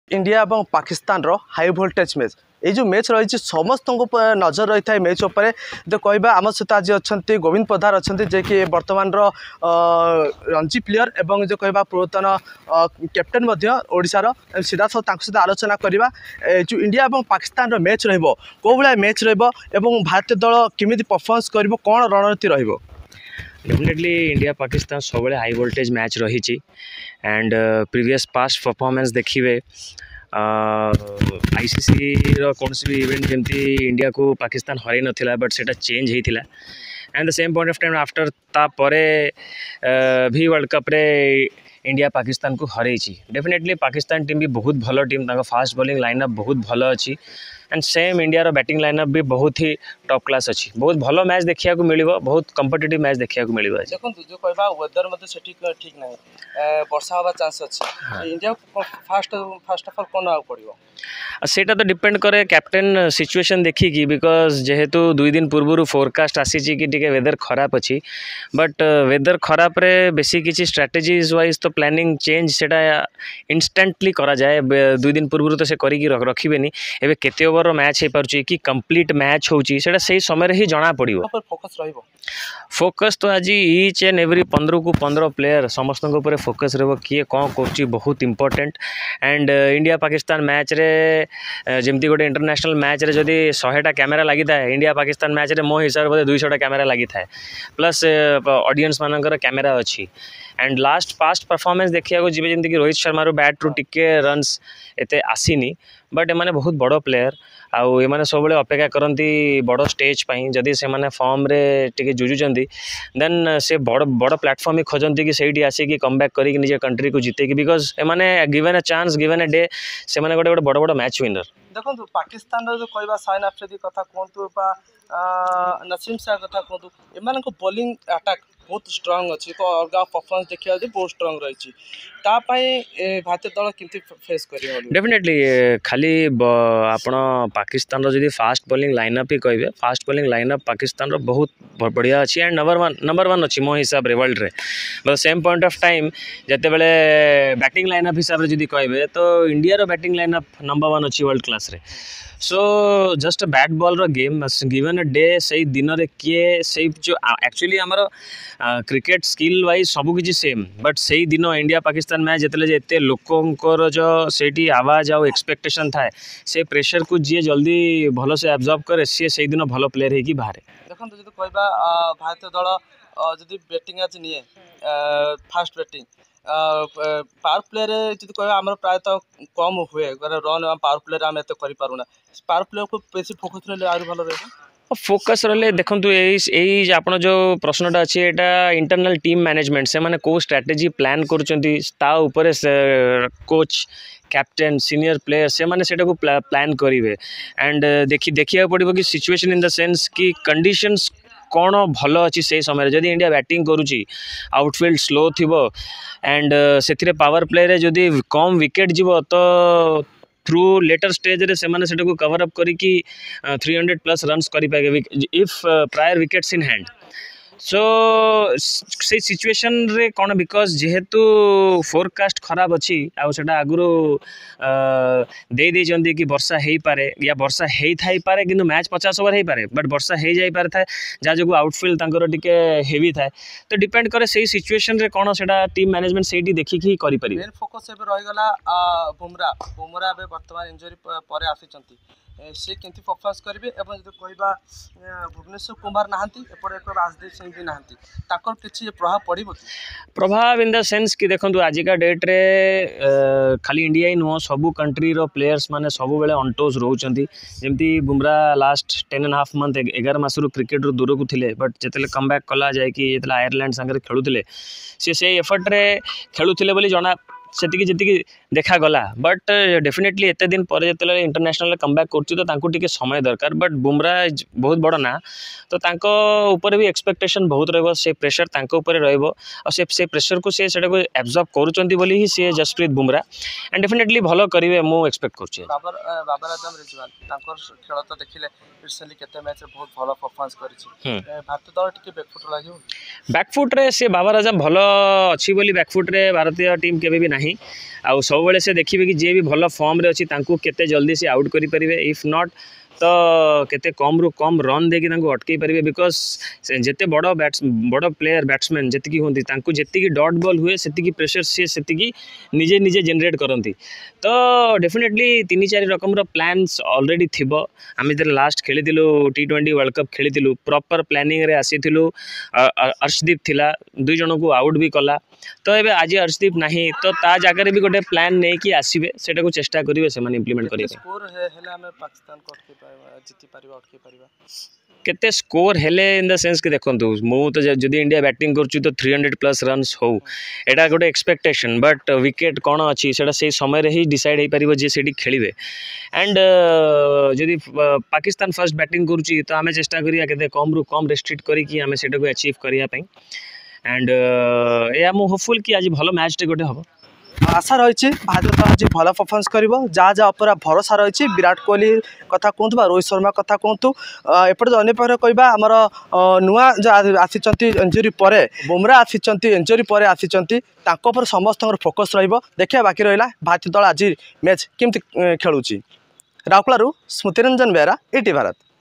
Asia Cup 2023 : ଭାରତ ଓ ପାକିସ୍ତାନ ମଧ୍ୟରେ ଆଜି ସଂଘର୍ଷପୂର୍ଣ୍ଣ ମ୍ୟାଚ ହେବ: ଗୋବିନ୍ଦ ପୋଦାର || Ind Vs Pak
Asia Cup 2023 : ଭାରତ ଓ ପାକିସ୍ତାନ ମଧ୍ୟରେ ଆଜି ସଂଘର୍ଷପୂର୍ଣ୍ଣ ମ୍ୟାଚ ହେବ: ଗୋବିନ୍ଦ ପୋଦାର || Ind Vs Pakଏସିଆ କପରେ ଆଜି ଭାରତ ଓ ପାକିସ୍ତାନ ମଧ୍ୟରେ ମ୍ୟାଚ ରହିଛି । ଅପରାହ୍ନ 3ଟା ସମୟରେ ଶ୍ରୀଲଙ୍କାର କାଣ୍ଡି ସହର ପାଲ୍ଲେକେଲ ଷ୍ଟାଡିୟମରେ ଖେଳାଯିବ ମ୍ୟାଚ । ଏସିଆ କପରେ ଭାରତ ଆଜି ପ୍ରଥମ ମ୍ୟାଚ ଖେଳିବ, ତାହା ପୁଣି ପାକିସ୍ତାନ ସହ । ସାରା ବିଶ୍ବର ନଜର ଏହି ମ୍ୟାଚ ଉପରେ ରହିଛି । ଆଜିର ମ୍ୟାଚ ନେଇ ଓଡିଶାର ରଣଜୀ ଖେଳାଳି ଗୋବିନ୍ଦ ପୋଦ୍ଦାର କହିଛନ୍ତି ଯେ, ପାକିସ୍ତାନର ବୋଲିଂ ବିଭାଗ ବହୁତ ଭଲ ରହିଛି ।